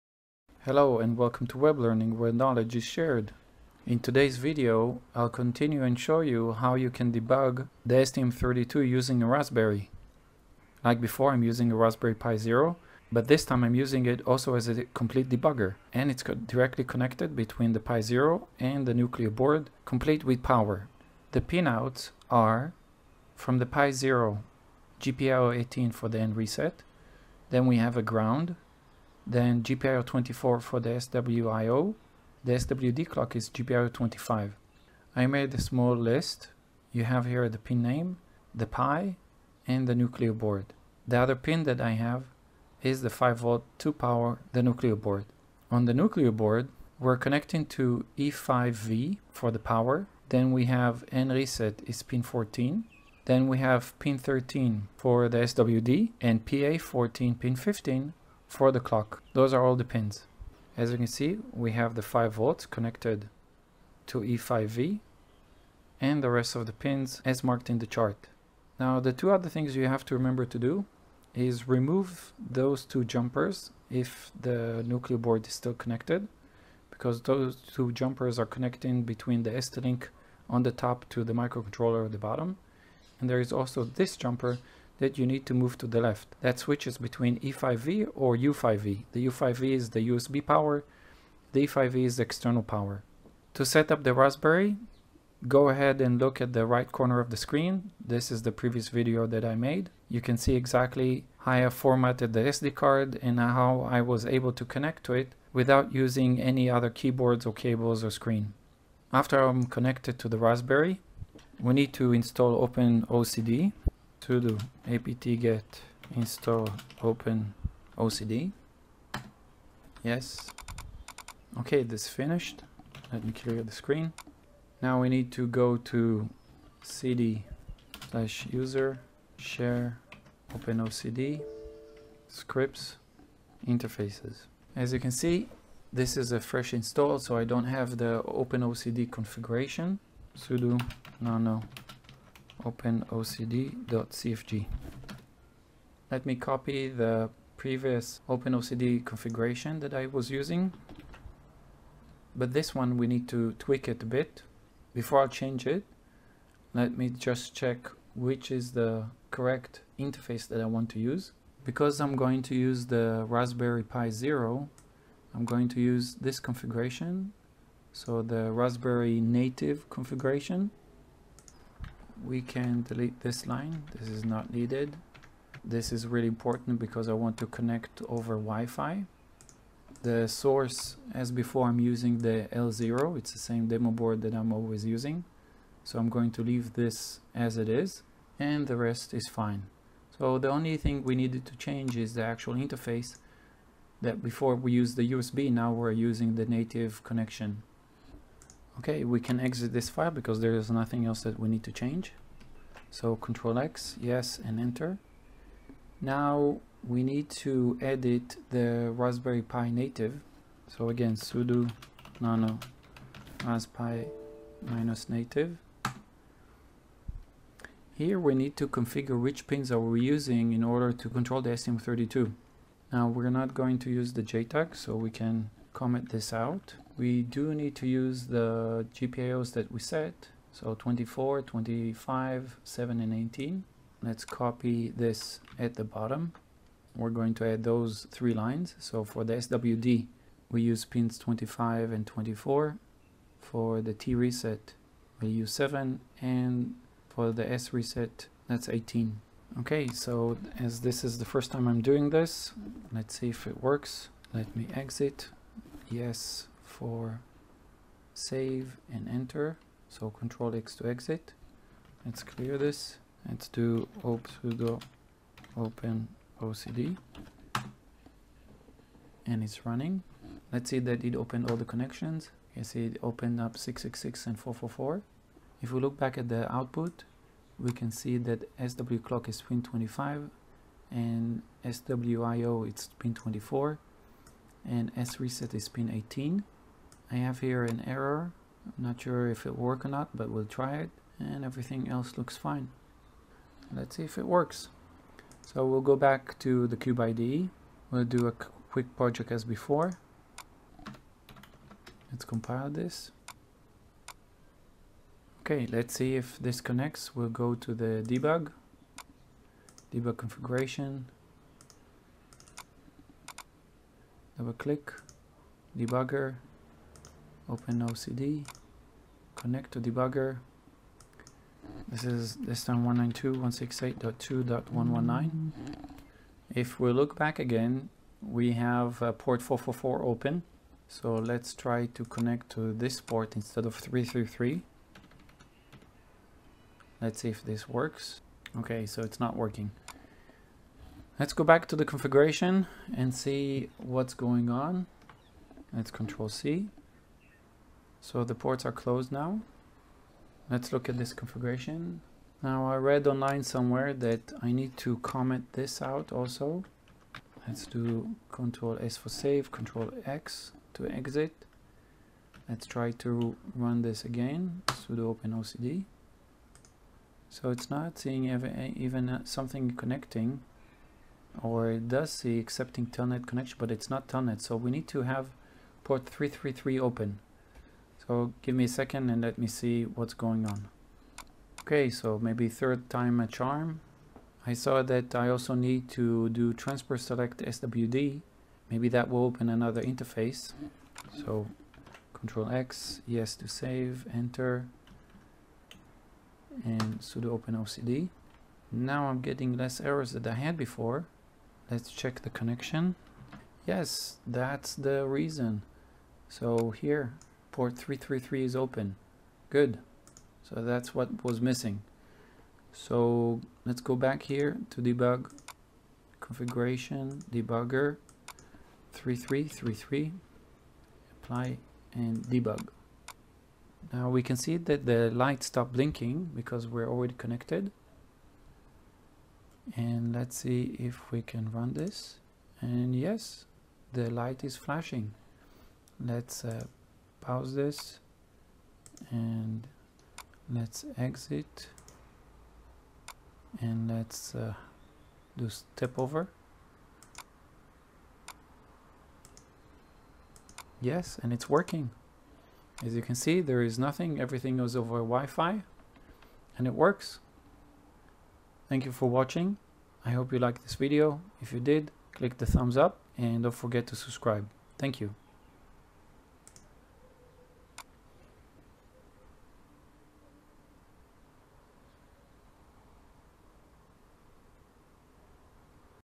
Raspberry Pi Zero W as Wi-Fi Debugger
In this video I will use the Raspberry Pi Zero W as a stand alone SWD debugger with no ST-Link. I will show the complete procedure ( with mistakes that I made ).

0:00 - introduction
0:21 - How to connect the Pi to the Nucleo
1:35 - setting the Raspberry Pi
1:55 - Installing Openocd
2:22 - setting the openocd.cfg file
3:34 - setting the raspberrypi-native.cfg file
5:00 - CubeIDE
6:27 - correct settings of the openocd.cfg

***  raspberrypi-native.cfg  ***
bcm2835gpio_swd_nums 25 24
bcm2835gpio_trst_num 7
bcm2835gpio_srst_num 18
*** opeocd.cfg   ***
source [find raspberrypi-native.cfg]
transport select swd
bindto 0.0.0.0
set WORKAREASIZE 0x2000
type of STM32
source [find ../target/stm32l0.cfg]
reset_config srst_only
adapter_nsrst_delay 100
adapter_nsrst_assert_width 100
init
targets
reset hal

Thanks.